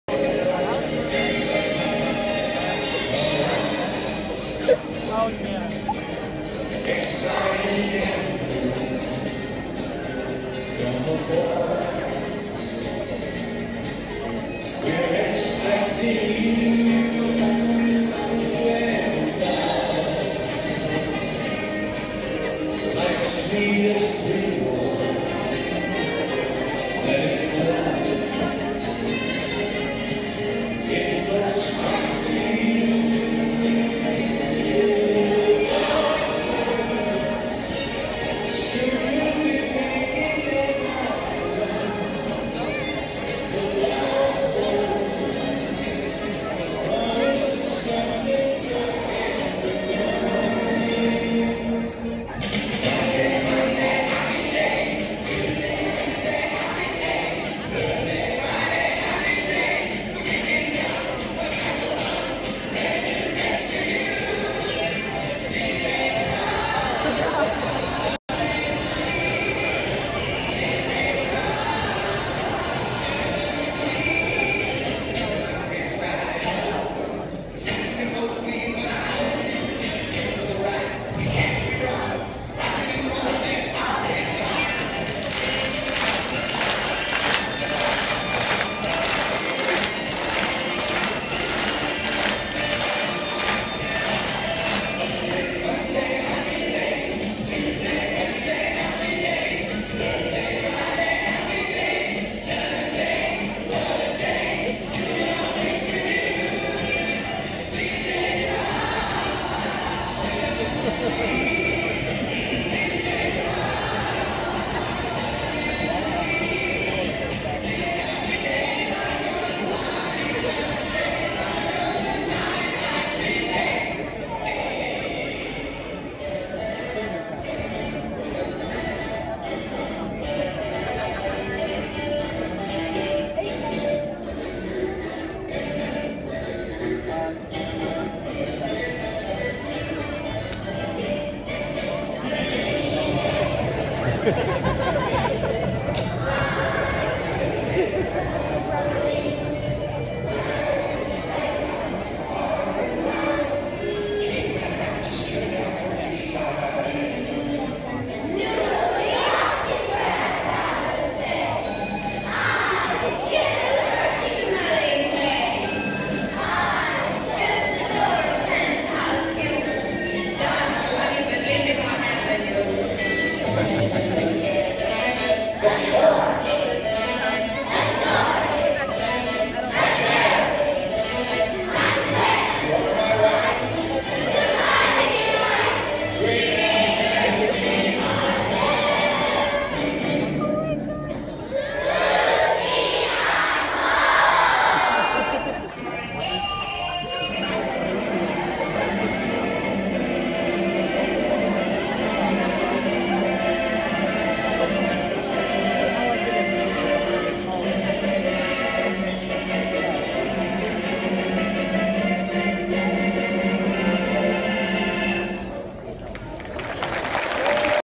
TV Land 2015 Hinton Elementary Spring Festiv
Hinton,OK.
Theme - TV Land. 70s and 80s favorite TV Shows
Love BOAT